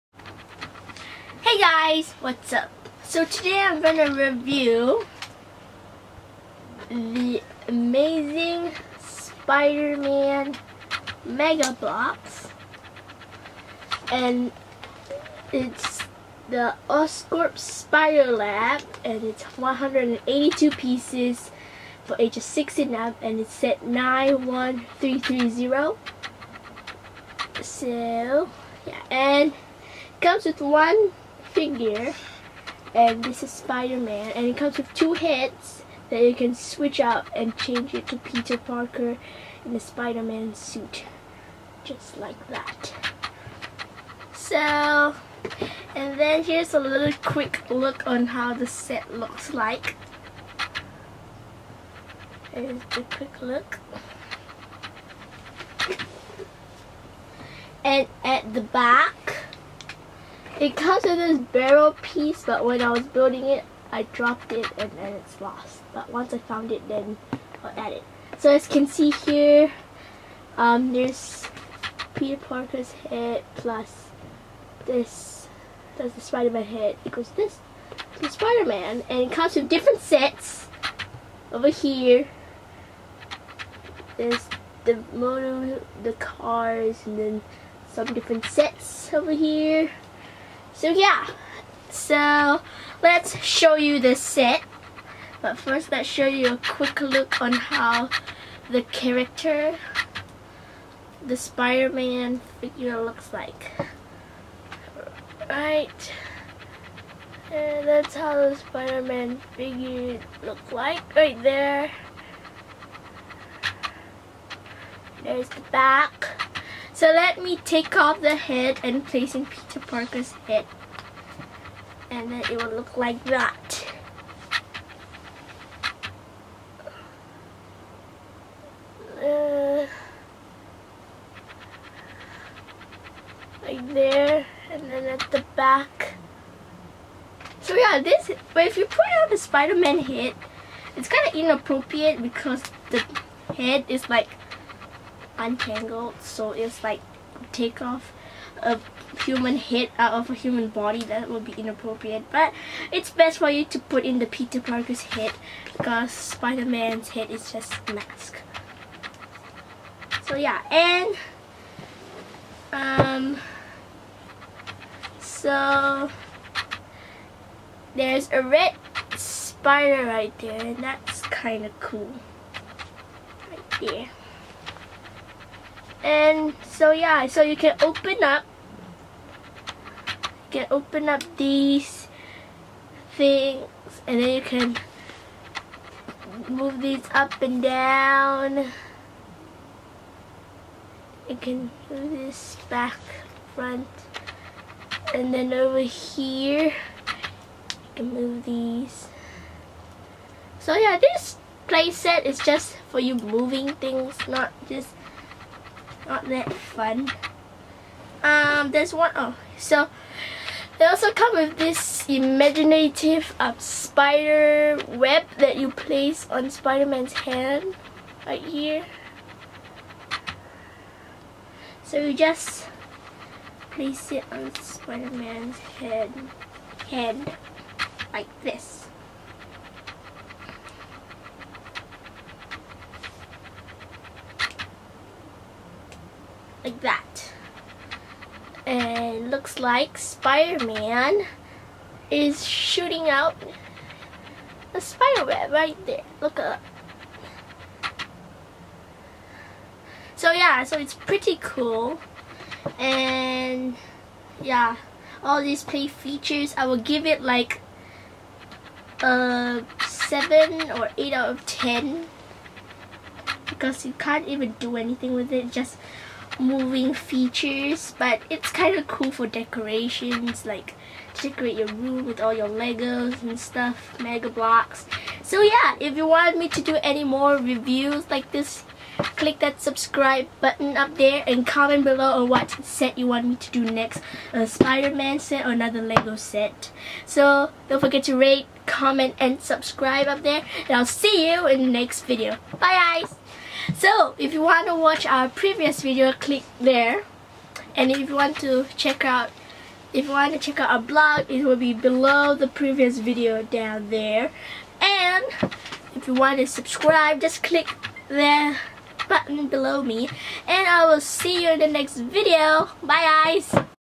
Review of The Amazing Spider Man MEGA BLOKS
Hey guys !
Check out our blog where we update you guys on our latest updates on our videos and channel:

SO here is another video for you guys ! We will be uploading videos on the weekends now as we are now back in school . :)